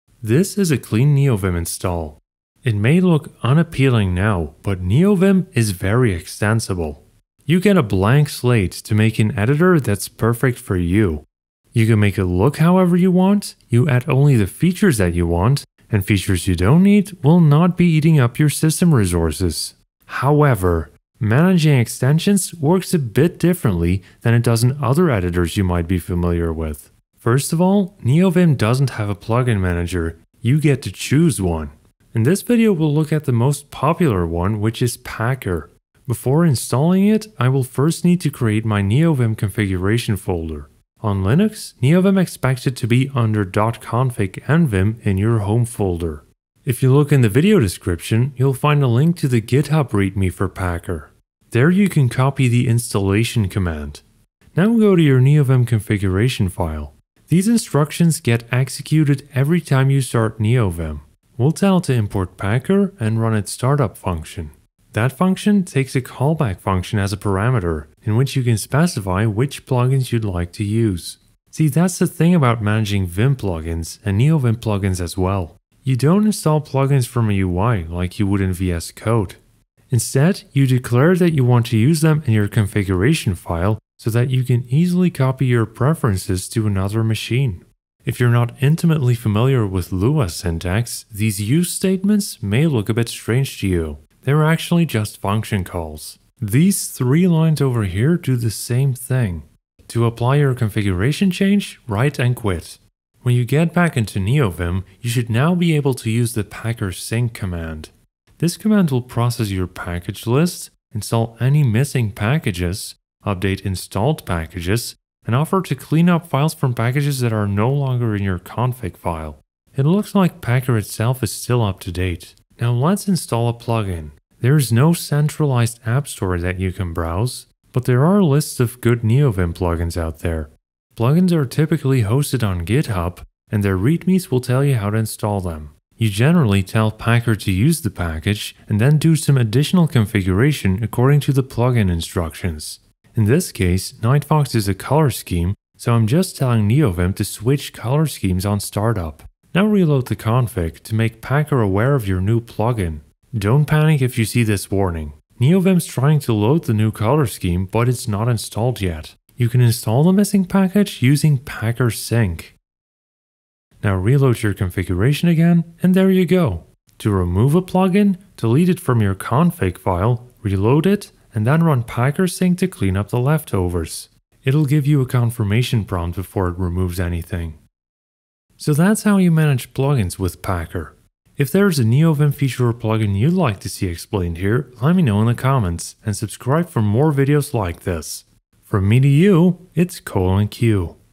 Managing Neovim plugins with Packer
Installing, updating and uninstalling extensions in Neovim works differently than in traditional editors like VSCode. Here's what you need to know.

 ~/.local/share/nvim/site/pack/packer/start/packer.nvim